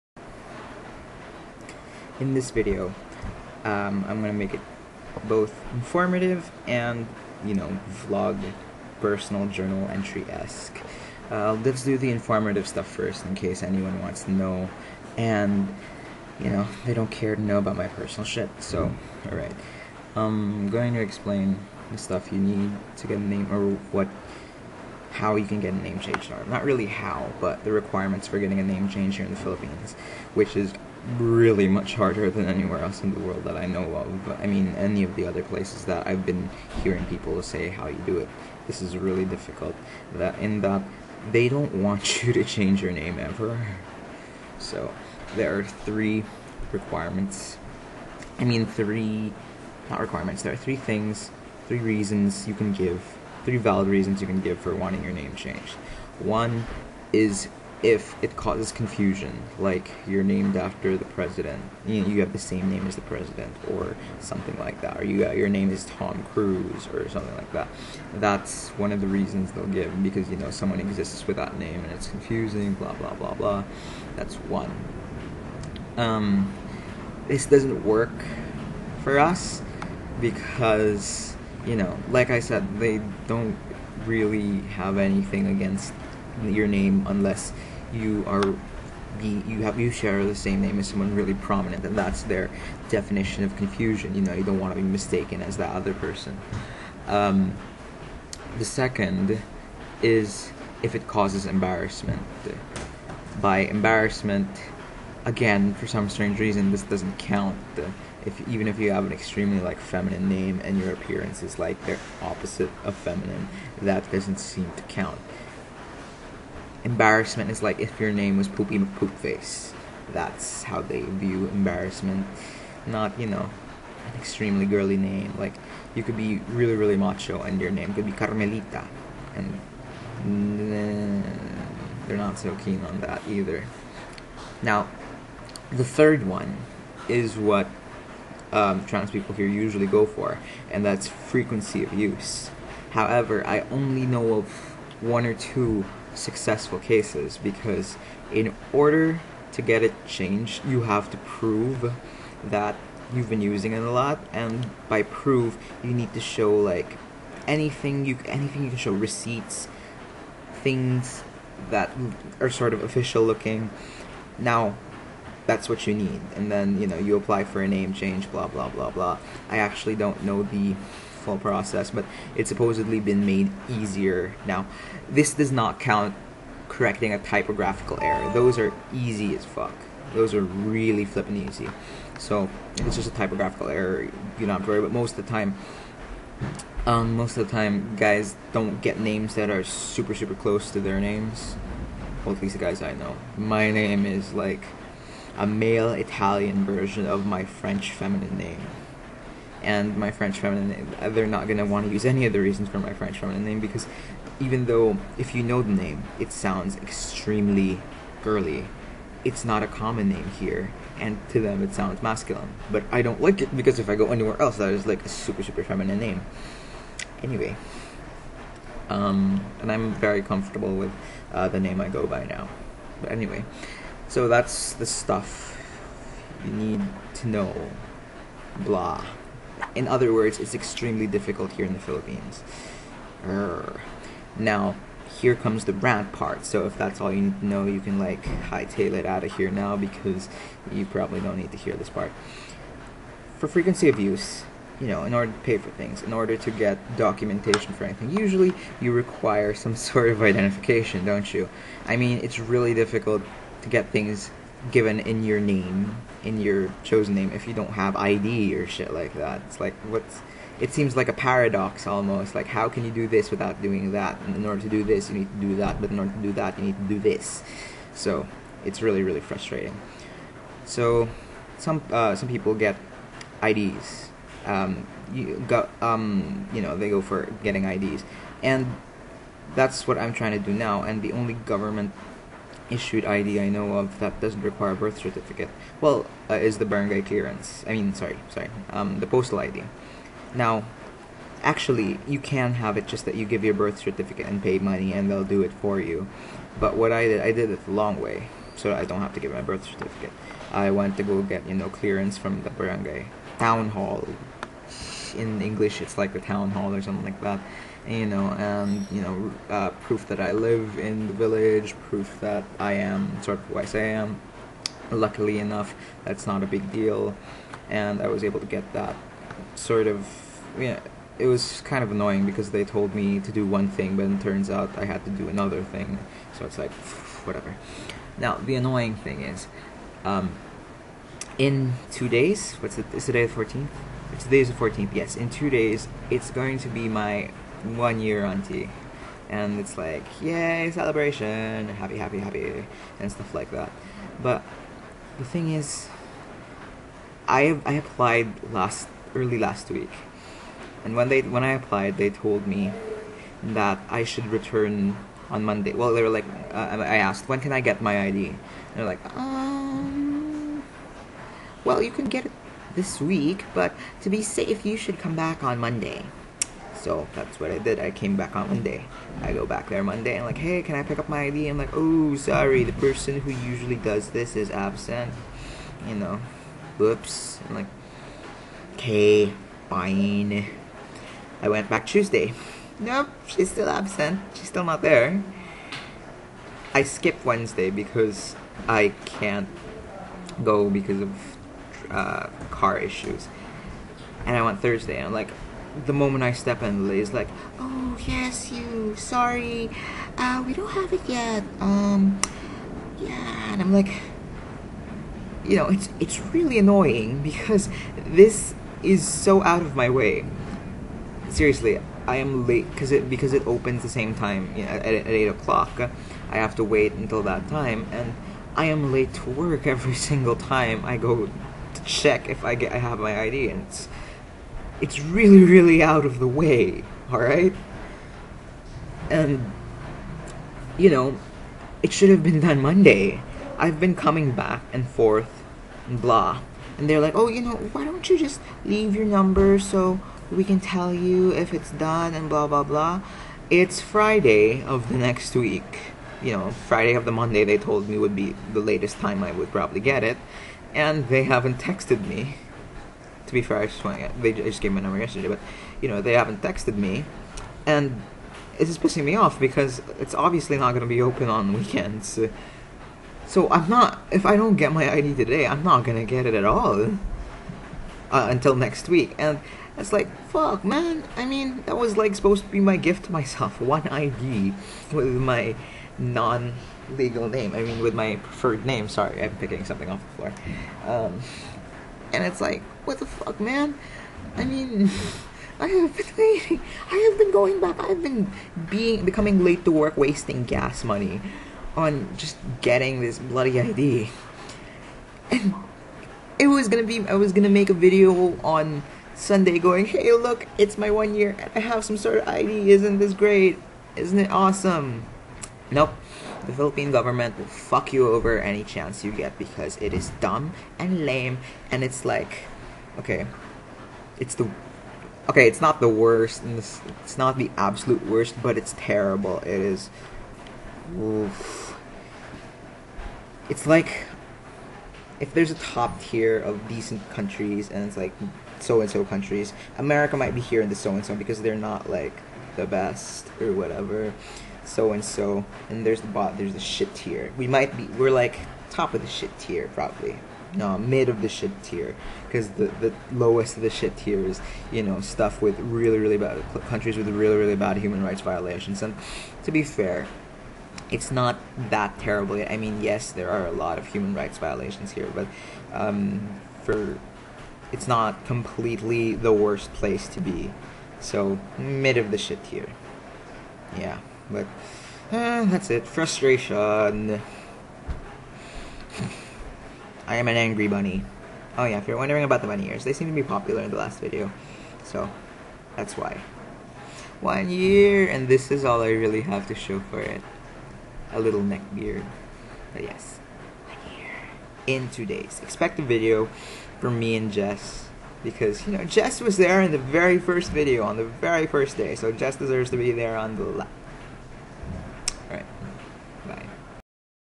Name Change in the Philippines + postal ID
The first half of the video is informative, the second is just a rant relating to the issue.